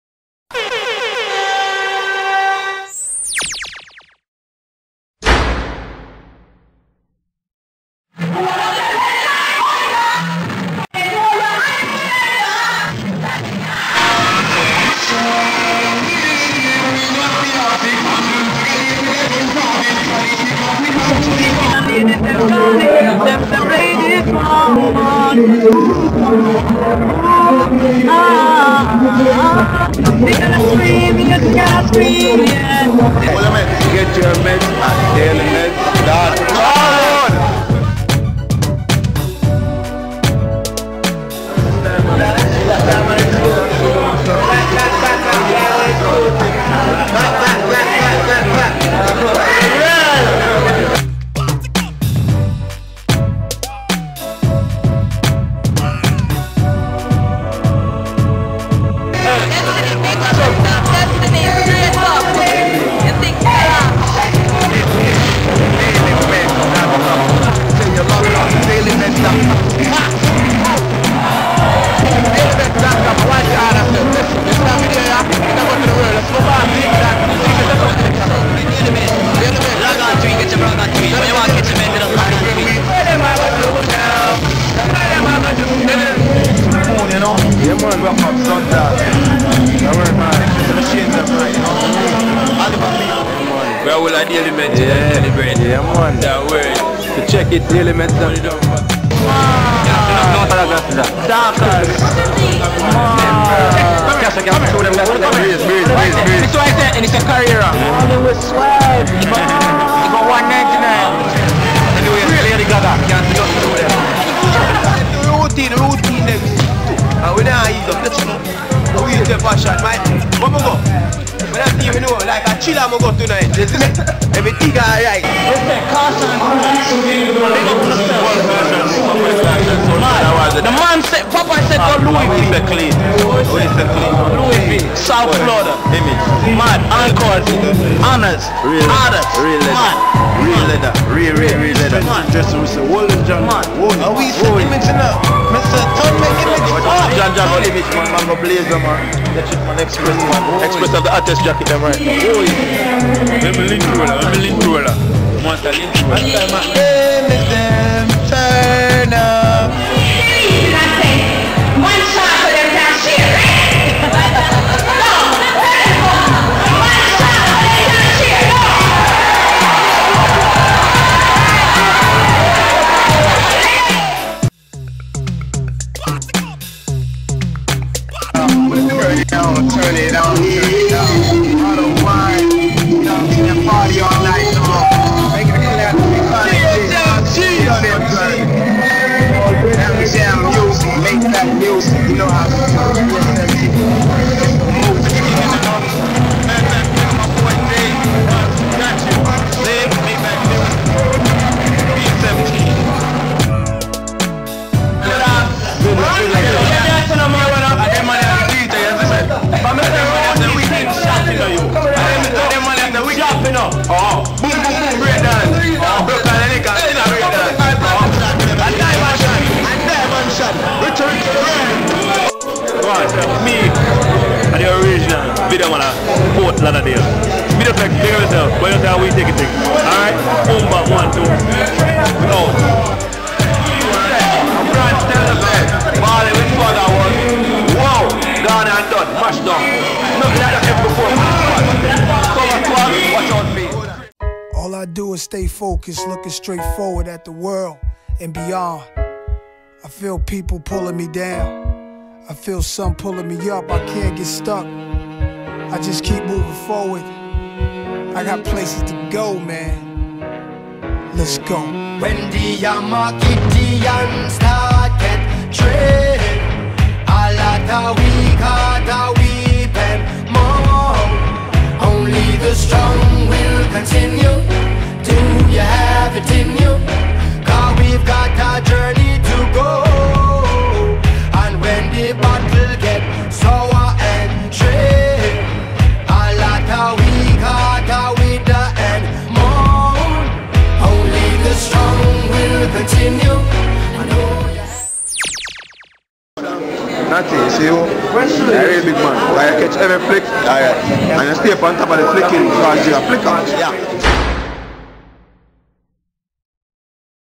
Beyond the Scenes - South Florida Dancehall Awards Part Two
Part two of beyond the scenes with BXD and DailyMedz.com of the 2nd Annual South Florida Dancehall Awards.

DailyMedz.com Exclusive